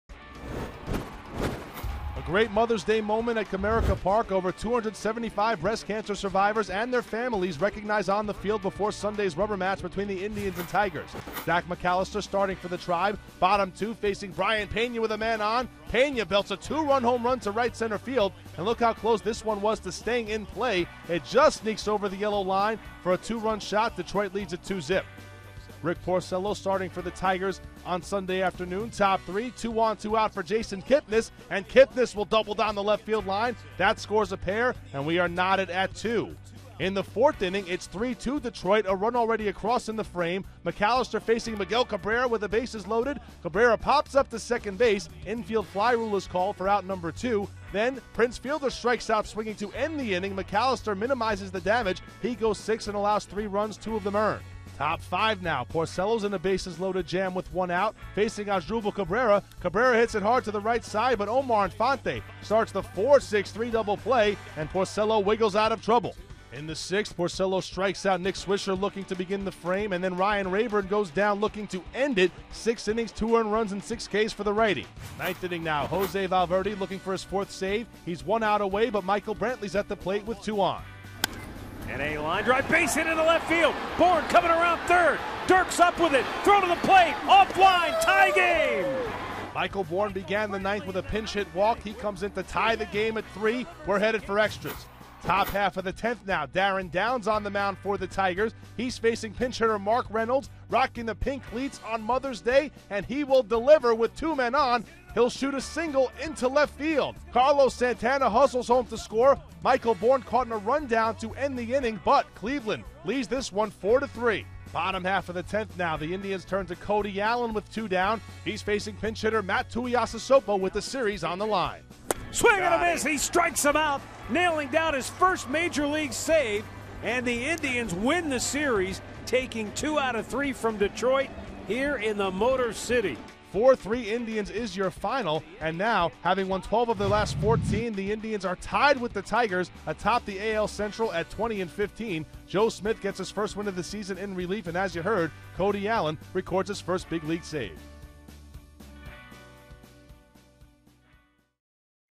2013/05/12 Recap: CLE 4, DET 3 - F/10
5/12/13: Indians rally to win in extras